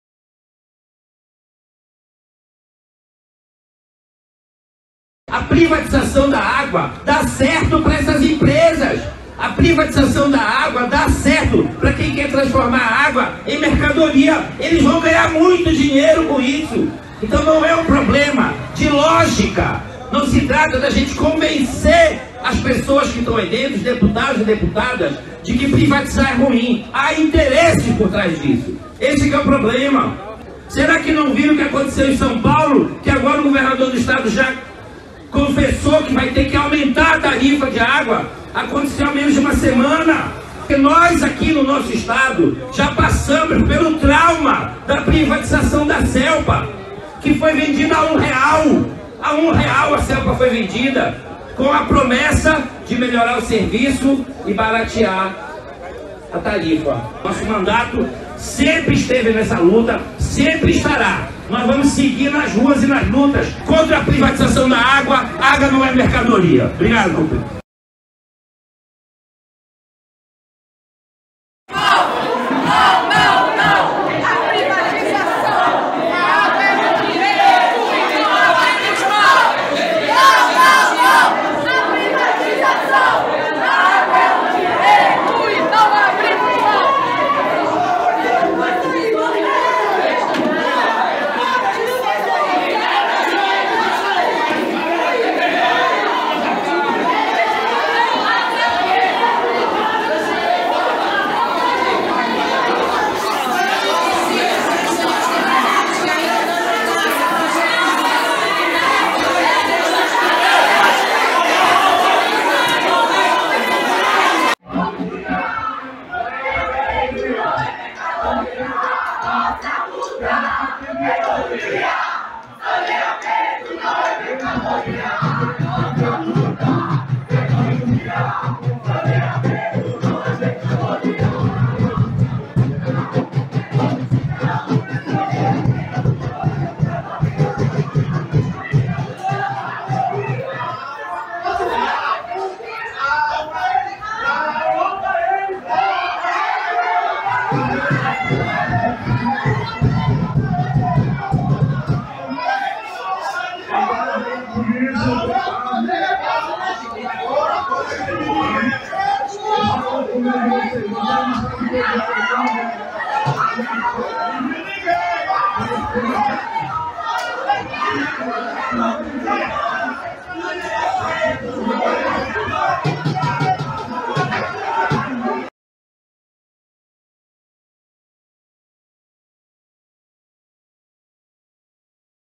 Patrimoniopublico
Hoje pela manhã, o Governador Helder Barbalho
conseguiu aprovar uma lei que possibilita a privatização da água no Pará!